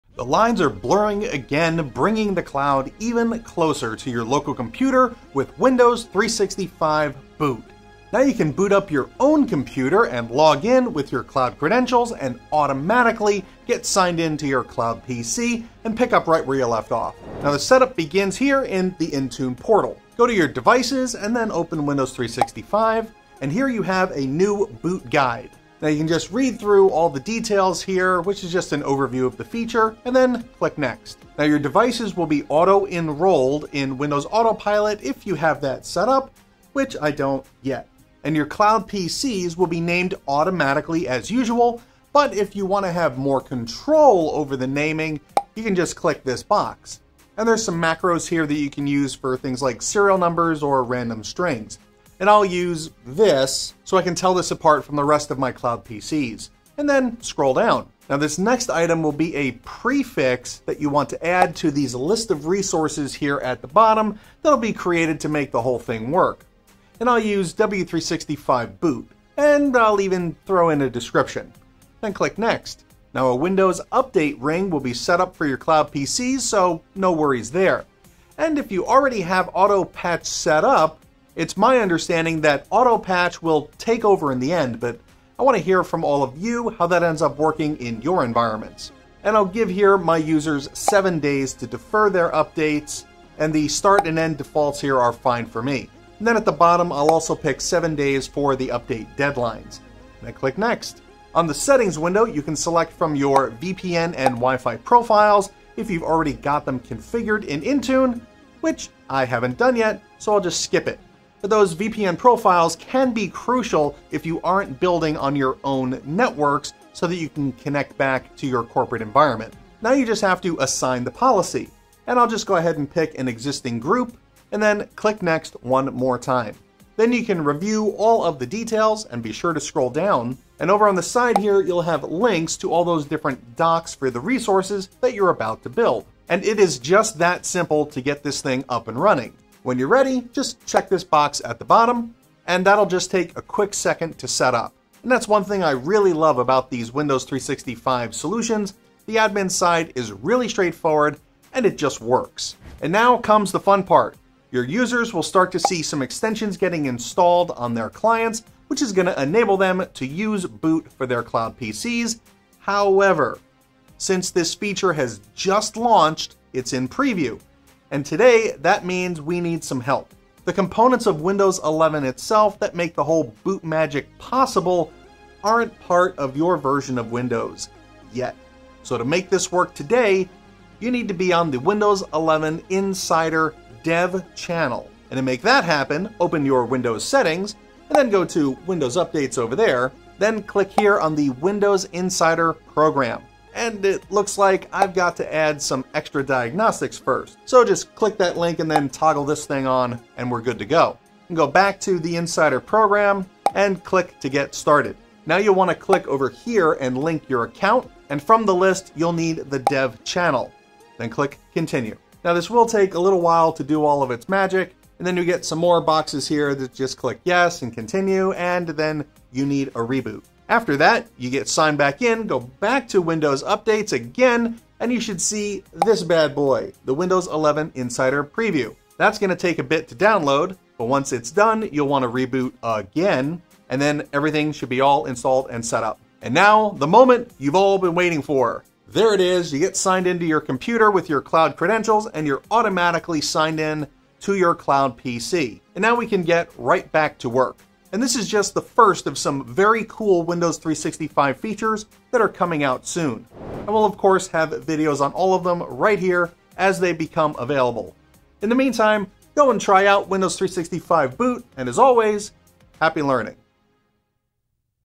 Windows 365 Boot Step By Step Guide
Windows 365 Boot allows you to log on to Windows 365 directly from your local computer without any additional logons or configuration, it just works as part of Windows 11 🔥AFTER THIS

00:00 Windows 365 Boot:
00:17 Win365 Boot Setup:
02:32 Local Computer Setup:
04:07 Wrap Up: